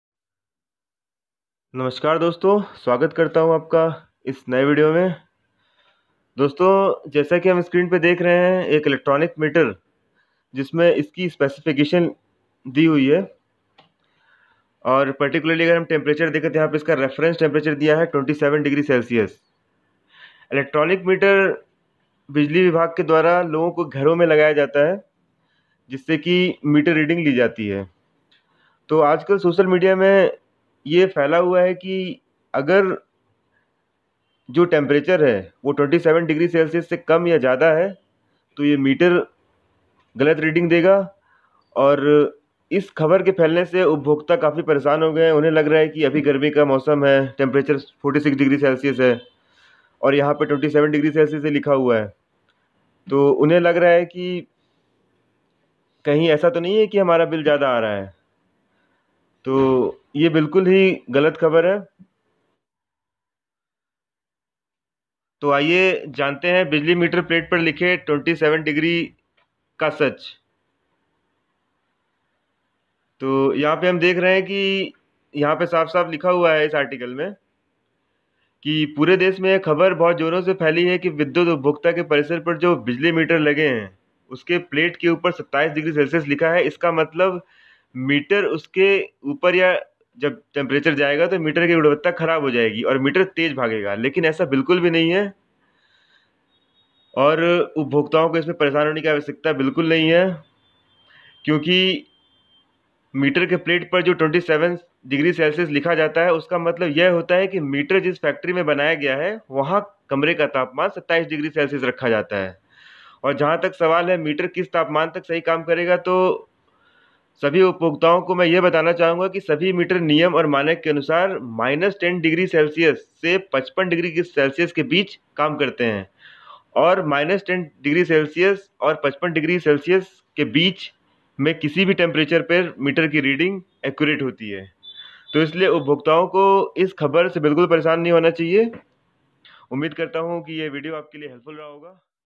बिजली मीटर मे लिखे 27 degrees Celsius का सच
मीटर मे 27 degrees celsius लिखे होने का सच
-10 से 55 degrees तक मीटर देता है सही रीडिंग